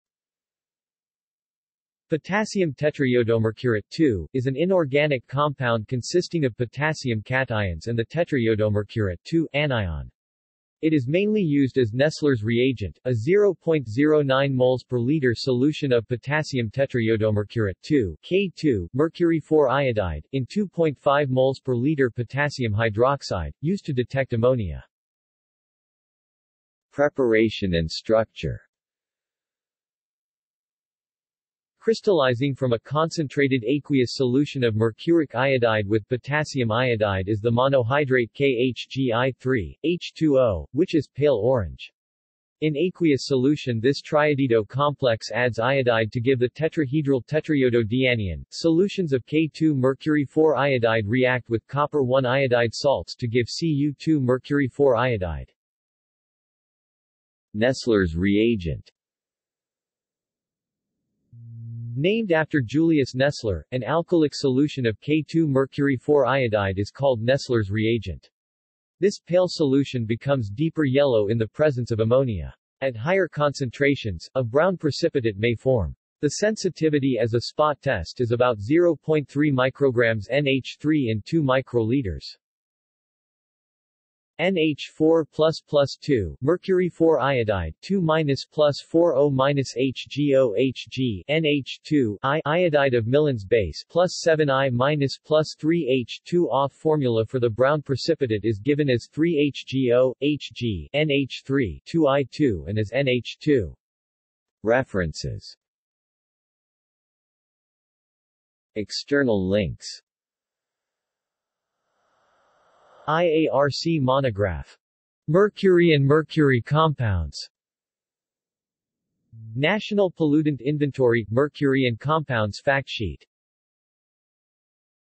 Potassium tetraiodomercurate(II) | Wikipedia audio article
This is an audio version of the Wikipedia Article:
Potassium tetraiodomercurate(II)

SUMMARY
Potassium tetraiodomercurate(II) is an inorganic compound consisting of potassium cations and the tetraiodomercurate(II) anion. It is mainly used as Nessler's reagent, a 0.09 mol/L solution of potassium tetraiodomercurate(II) (K2[HgI4]) in 2.5 mol/L potassium hydroxide, used to detect ammonia.